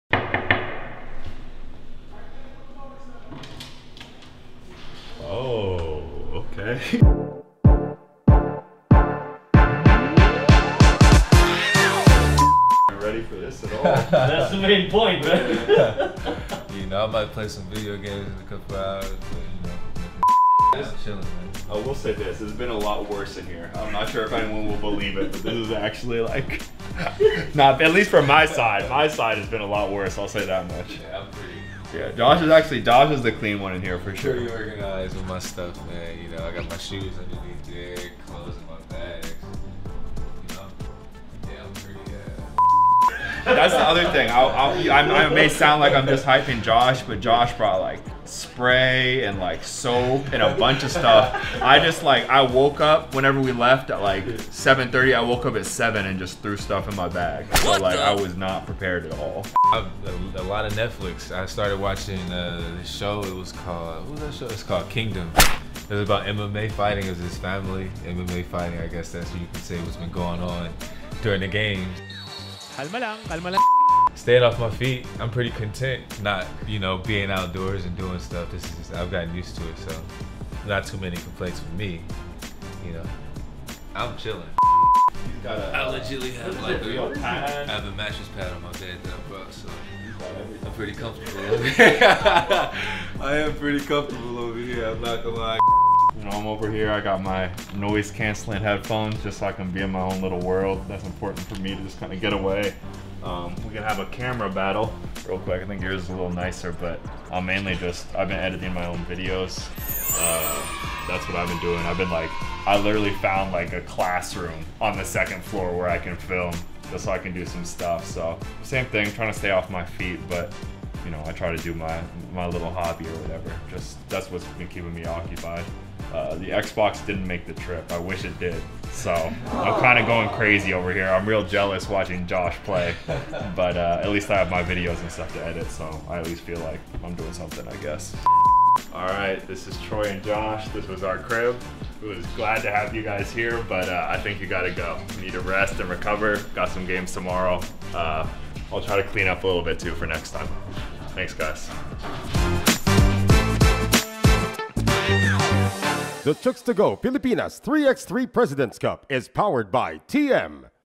Inside the Chooks Calambubble with Troy Rike and Joshua Munzon
Kamusta nga ba ang buhay ng ating mga ManokNgBayan sa loob ng Calambubble?

Nandito sina Troy Rike at Joshua Munzon para ipakita sa atin kung ano ang kanilang sitwasyon sa loob mismo ng kanilang kwarto.